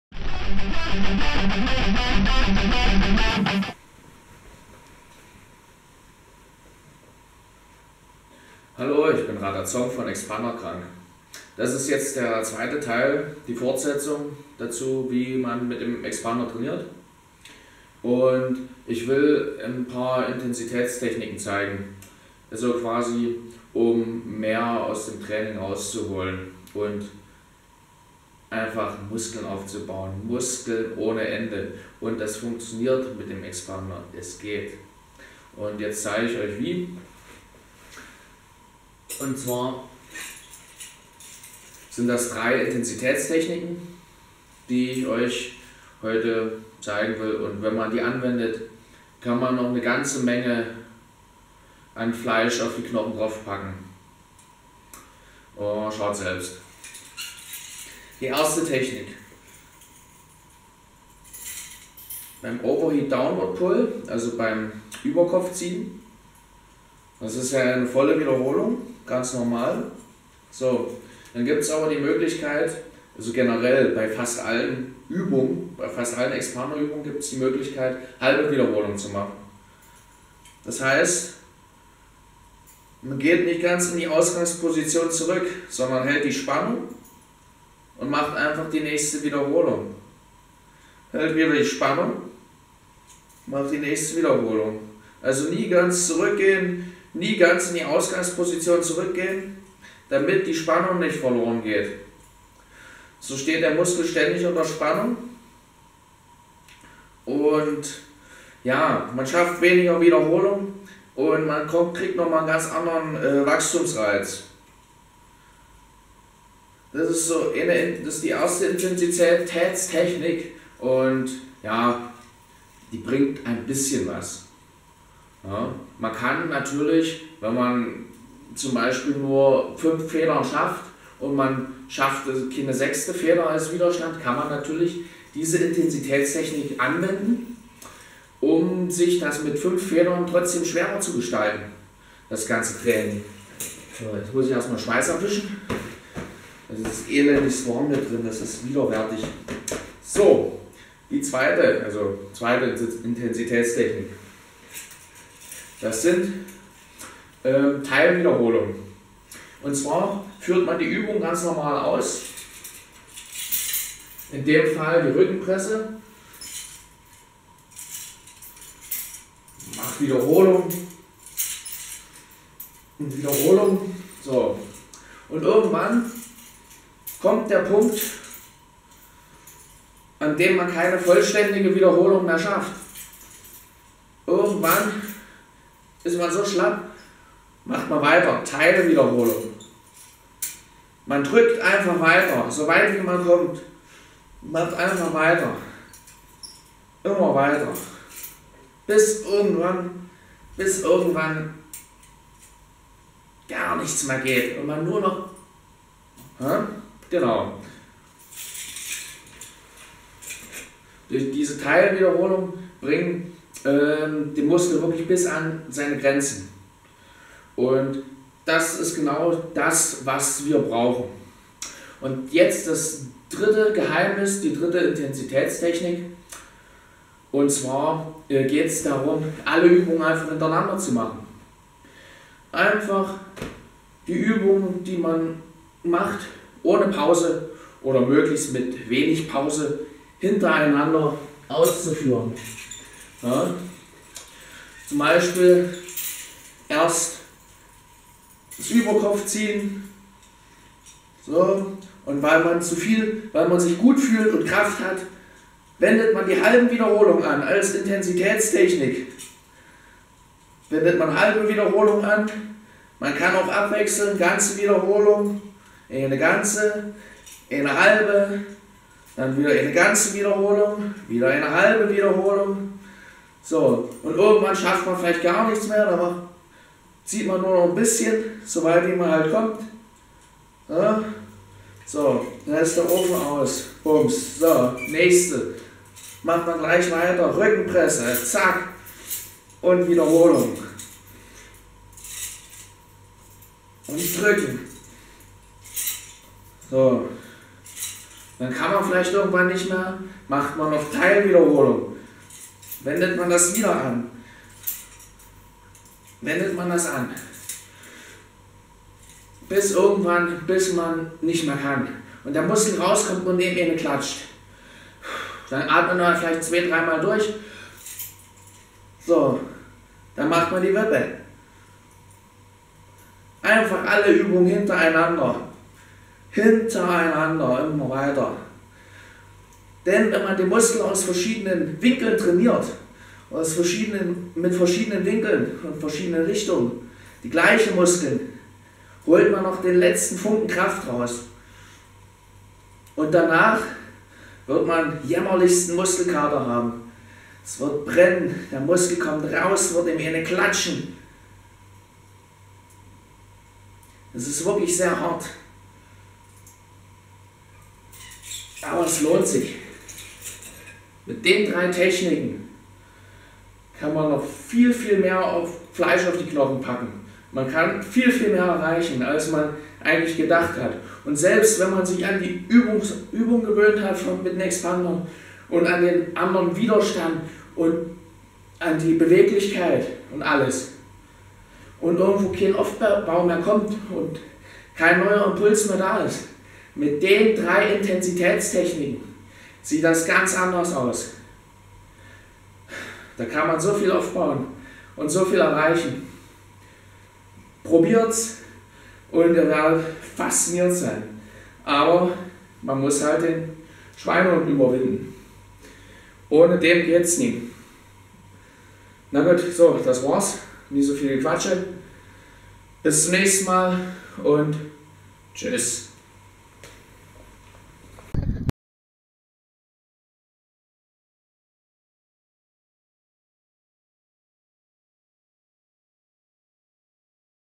Gain More Muscles Using Chest Expanders (RataZong)
How to train with chest expanders?
Rata Zong gives useful trainings technics you can use to increase your strength with strands even further!

--- Deutsch ---

Wie soll man mit dem Expander trainieren?
Rata Song erklärt 3 wichtige Intensitätstechniken um mit dem Expander noch mehr Fleisch und Kraft auf die Knochen zu packen.

Dir hat es gefallen?